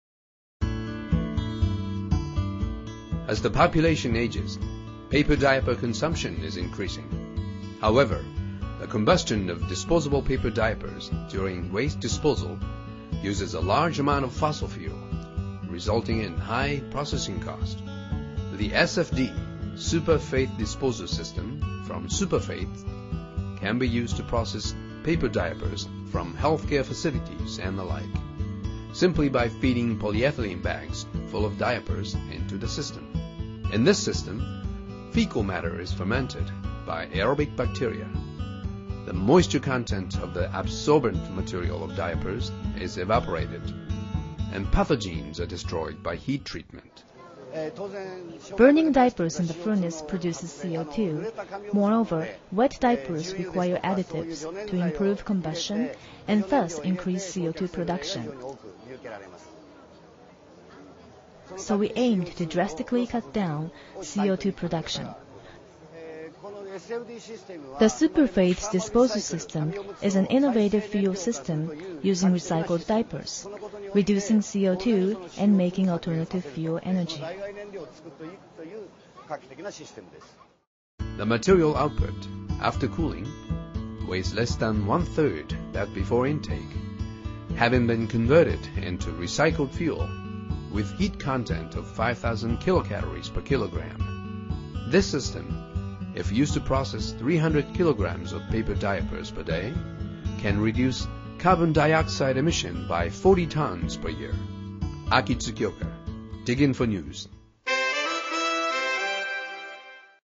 Paper diapers used as fuel : DigInfo
Burning diapers in the furnace produces Co2, moreover, wet diapers require additives to improve combustion and thus increase Co2 production. So we aimed to drastically cut down Co2 production.
The Super faiths disposer system is an innovative fuel system using recycled diapers, reducing Co2 and making alternative fuel energy.